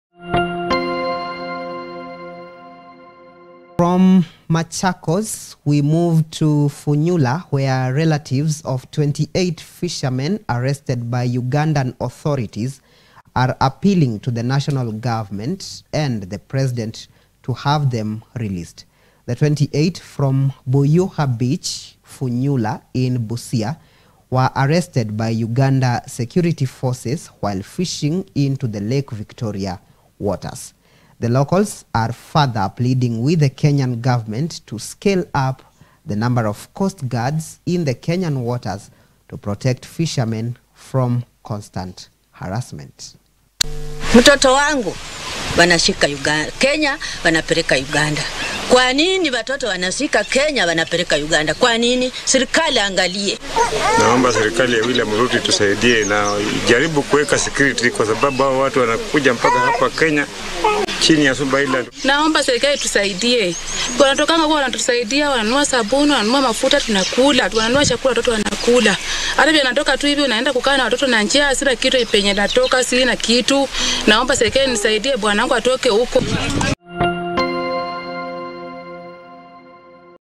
28 fishermen in Funyula arrested by Ugandan authorities
Relatives of 28 fishermen arrested by Ugandan authorities are appealing to the National Government and the President to intervene and have them released by Uganda authorities.